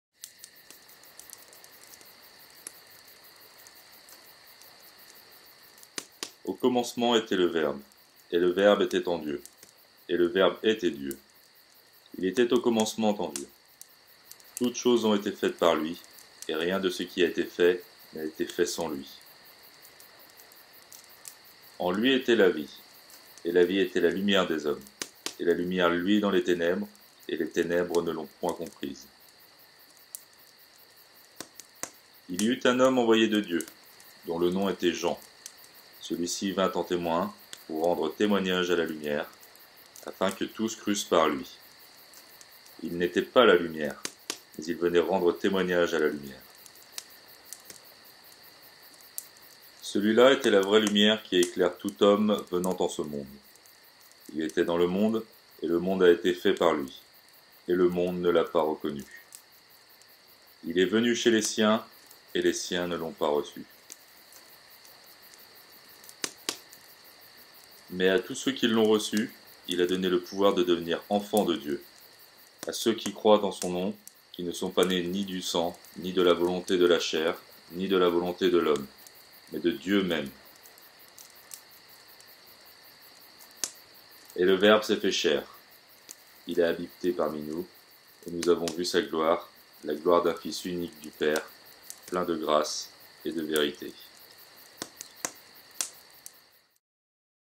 📖 PROLOGUE DE L'ÉVANGILE SELON SAINT-JEAN 📖
L'évangile selon Saint-Jean est une prière d'une protection vraiment extraordinaire, certainement due à son égrégore chargé depuis des siècles...

🎚 Ouvrages de référence 🎚